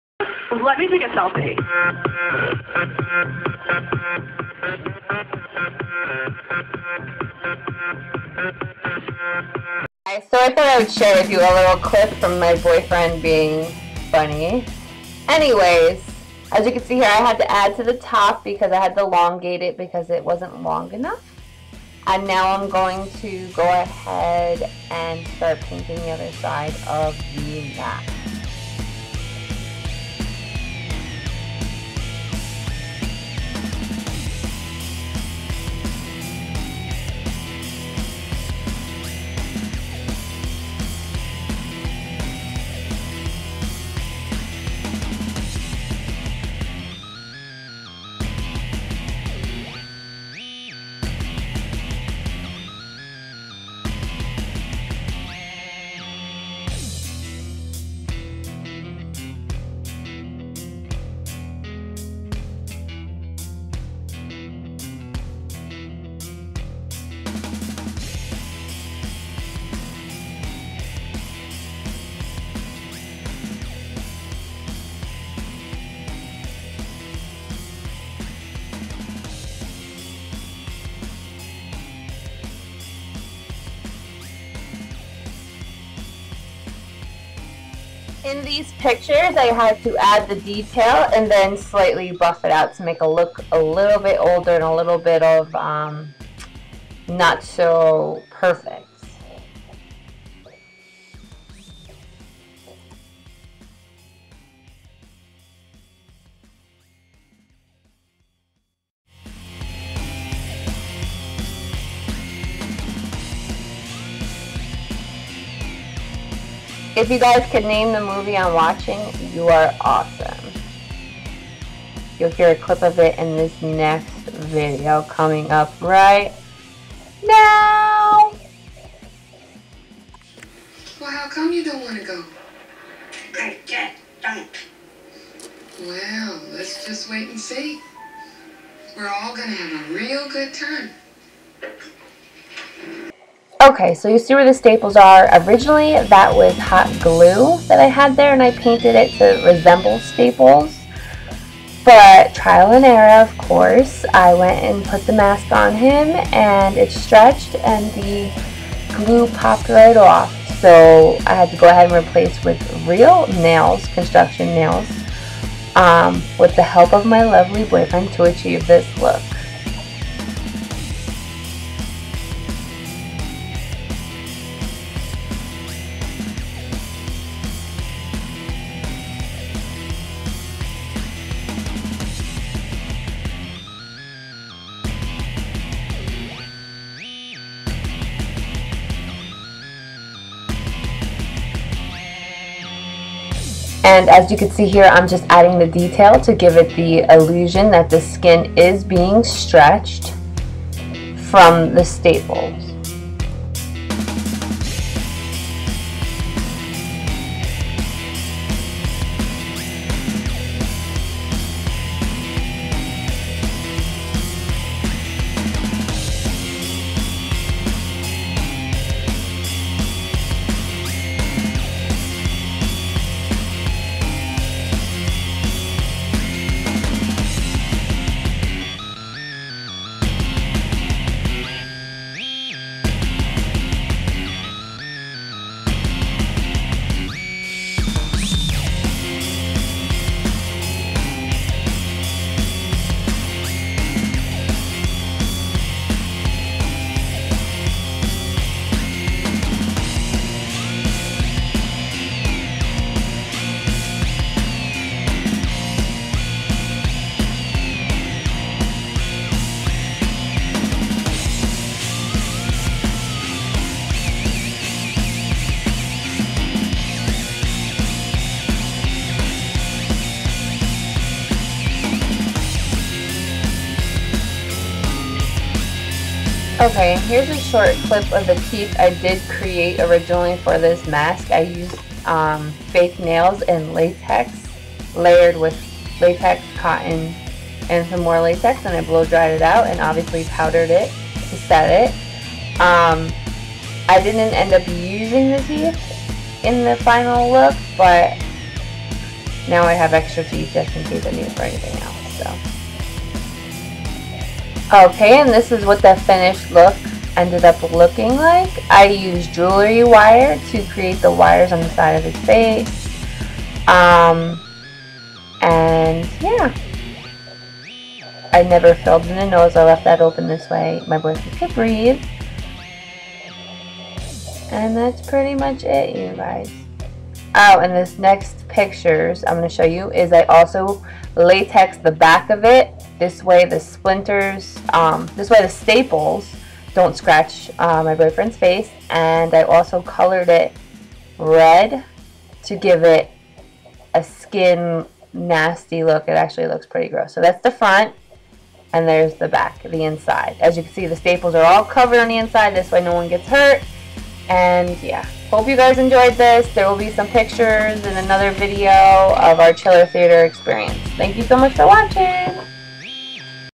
(Part Three) The new Joker 52 Mask
Hey guys so here are the final touches on my mask I made! Hope you guys like!!!